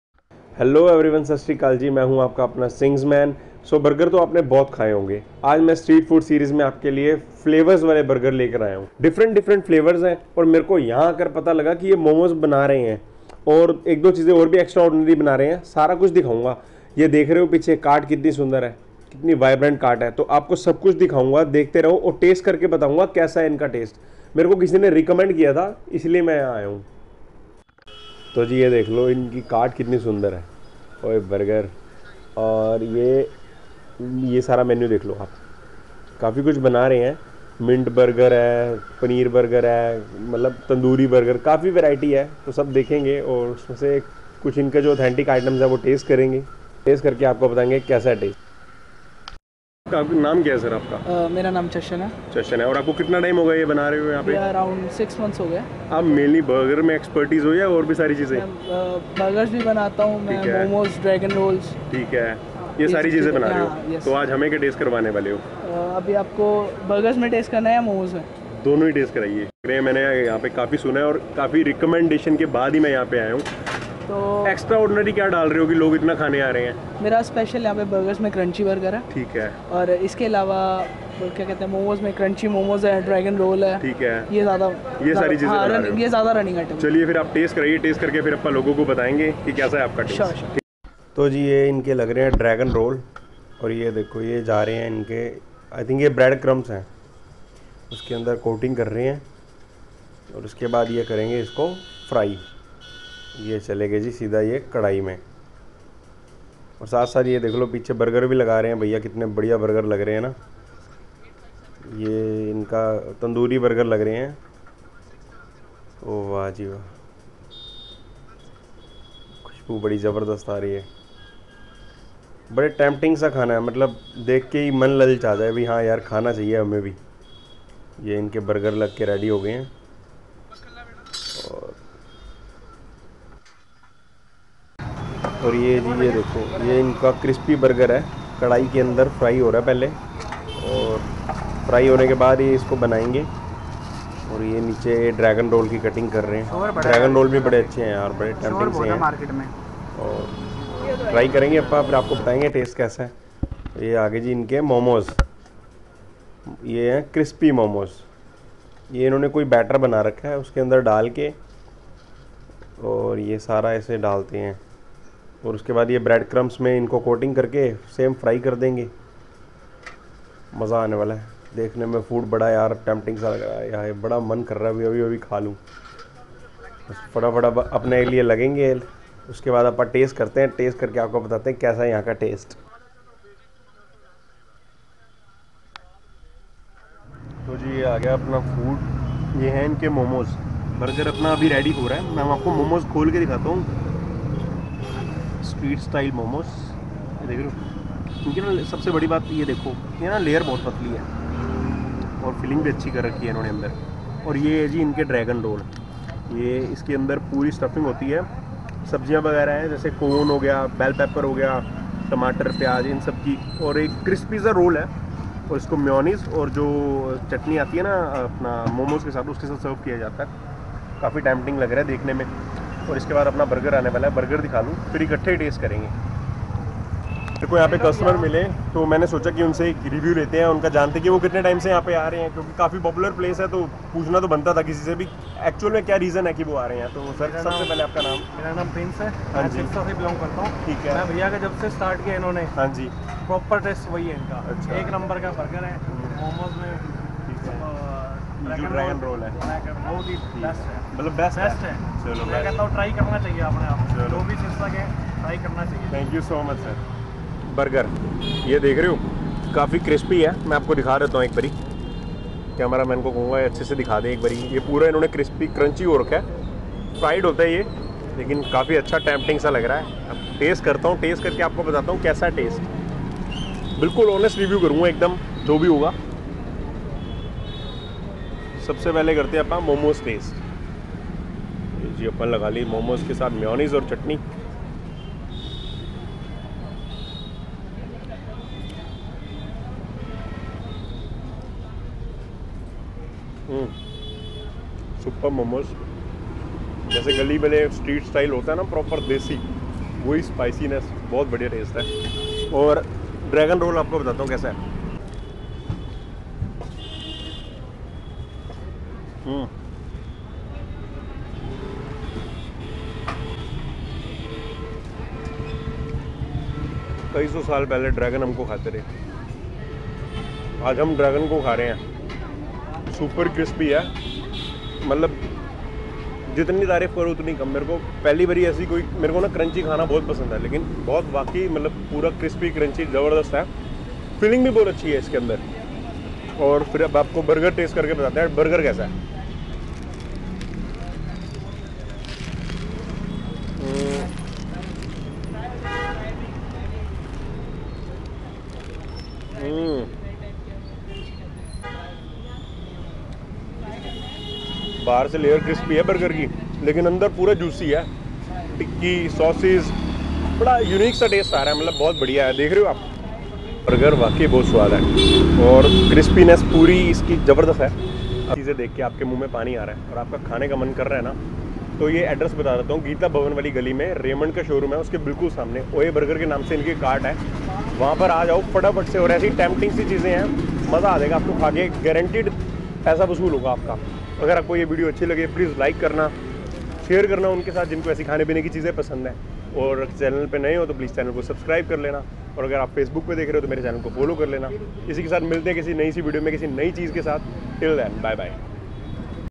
oye burger khaya kya | Street food series sirsa | indian street food | Ep17
Address: geeta bhawan wali gali sirsa opp raymond showroom,near bhalla tailor,sirsa (hry)